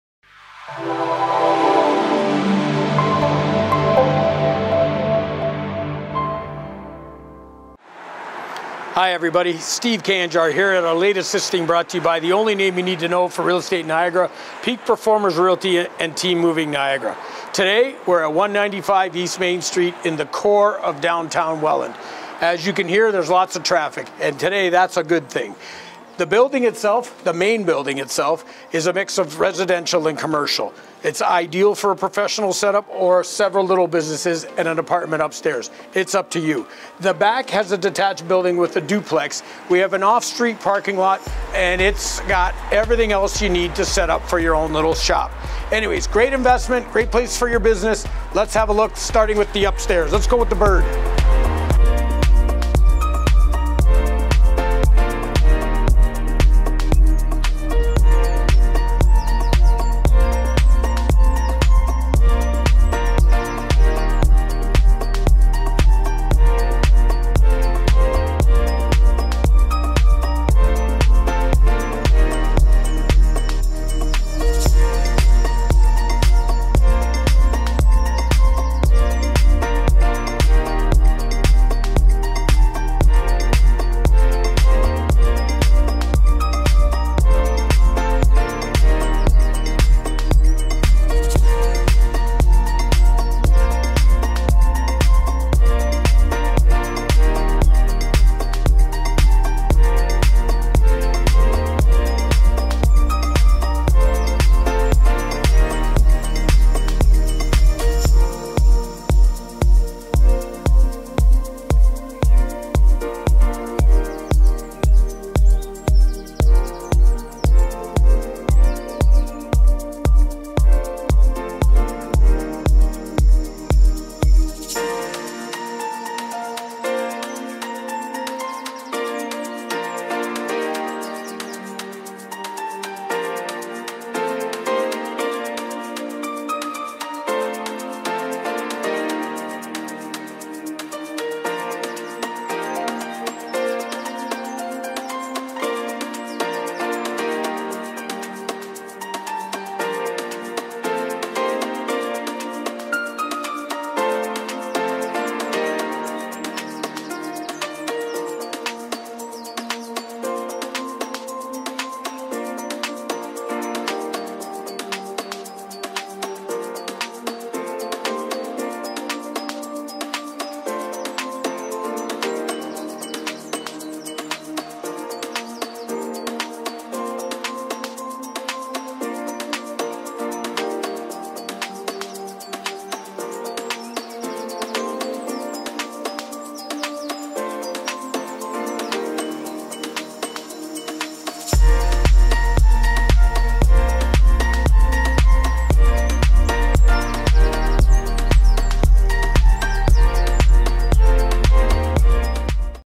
195 East Main St Welland ON Canada
Historic mixed use property located steps away from beautiful Merrit Island and the recreational Welland Canal. Main building is ideal spot for those professional offices (currently home to an nice mix of businesses) which includes individual offices, stunning 3rd floor board room and common areas - with bonus residential unit potential.  The back building is residential with 2 apartments.  Classic characteristics range from original woodwork and hard wood floors.  Ample off street parking in the parking lot included with the property.  A great home for your business or investment.